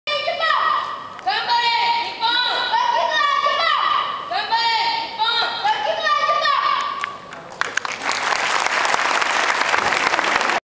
"GAMBARE NIPPON" shouted loudly @ "SYMPATHY for JAPAN" Charity COncert - 9 Apr 2011 JCC
2 Kids shouted "GAMBARE NIPPON" repeatedly and loudly on the ocassion of "SYMPATHY for JAPAN" Charity Concert - 9 April 2011 to give moral support to all Japan Tsunami Victims. Japan has helped Indonesia a lot during Aceh Tsunami in 2004. Now it is time to return the favor.